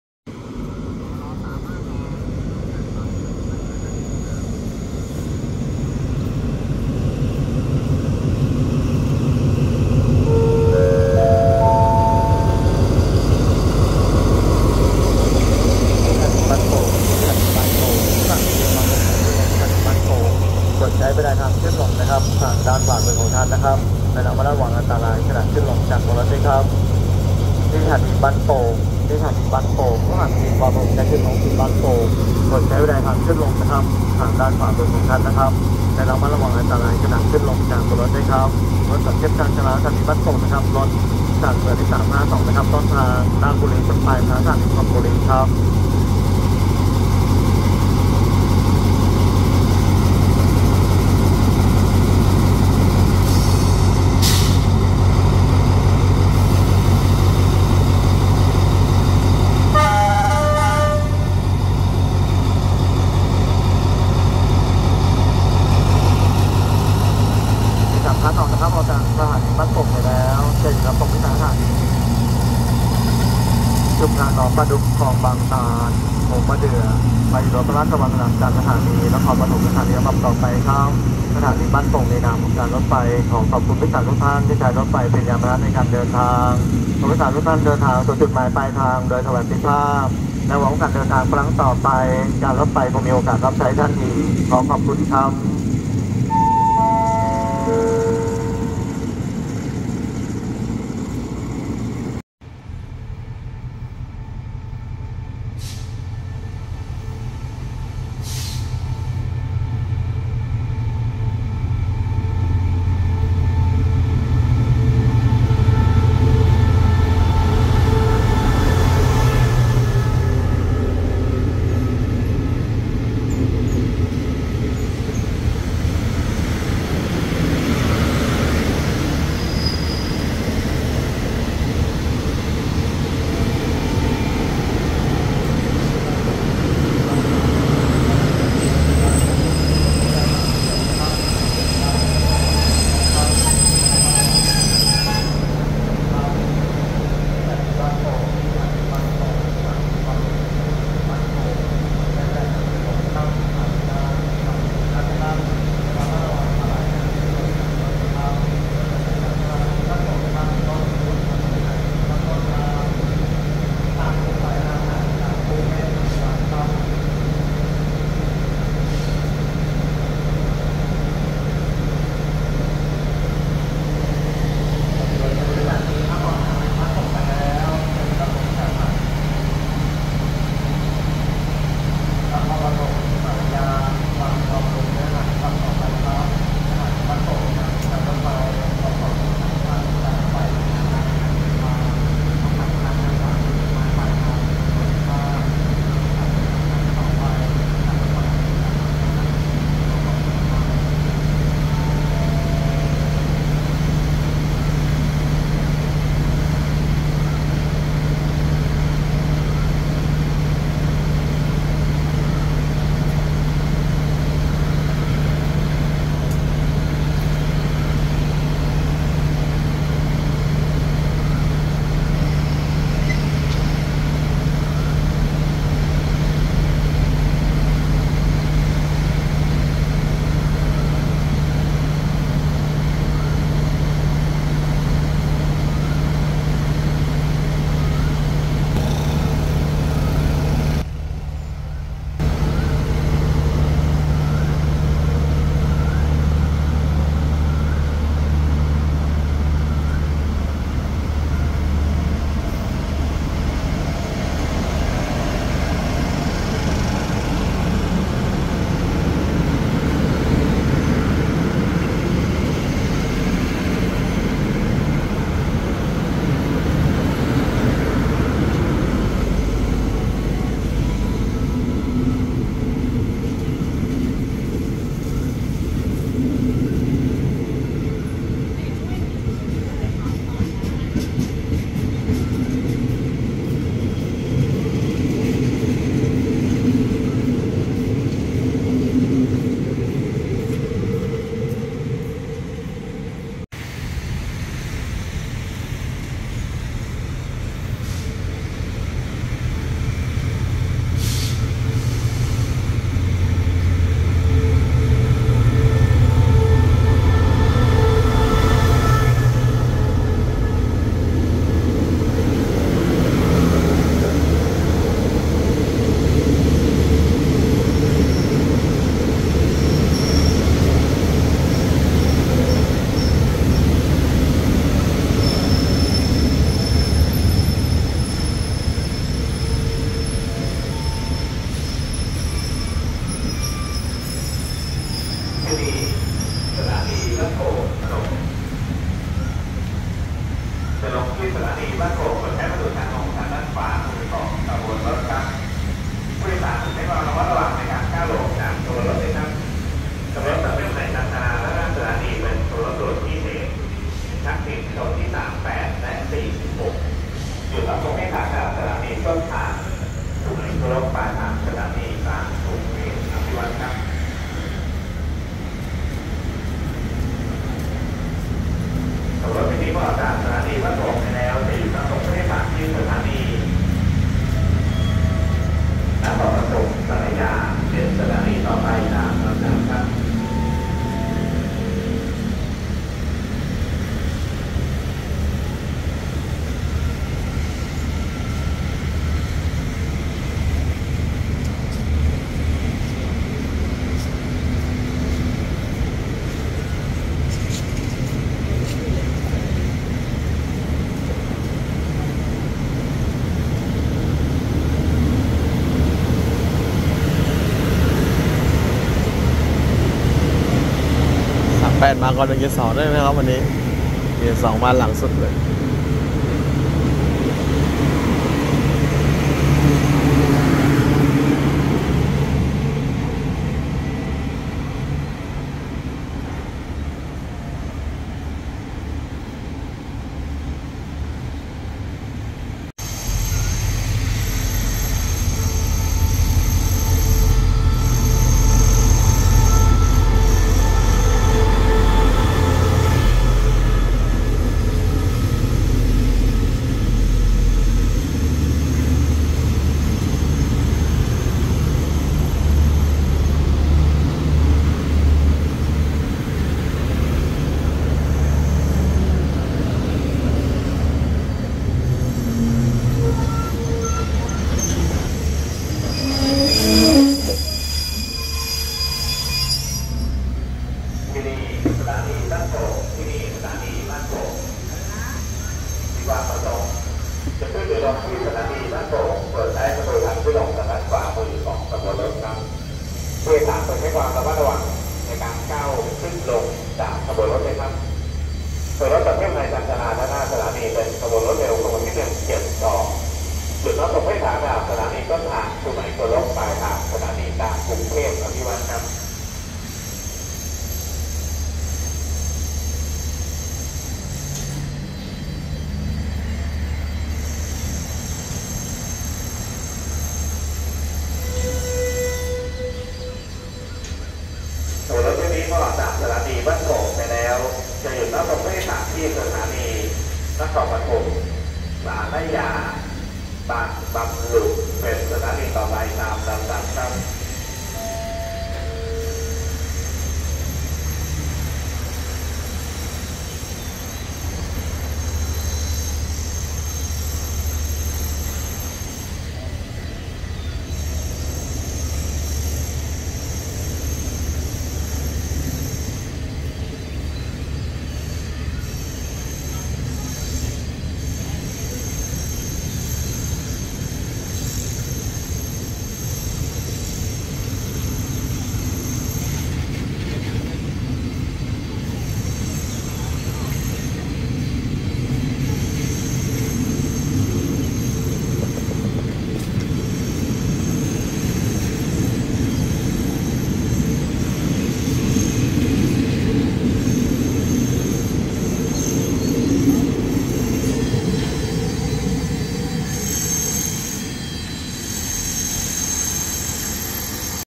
ชานเมือง352 ด่วน84 เร็ว172 ด่วนพิเศษทักษิณ38/46 จอดบ้านโป่ง 30 มกราคม ค.ศ. 2024
Comuter train 352 Ratchaburi - Thonburi
Express train 84 Trang - Krungthep Aphiwat
Rapid train 172 Su-ngai Kolok - Krungthep Aphiwat
Thaksin Special Express train 38/46 Sungai Kolok/Padang Besar - Krungthep Apiwat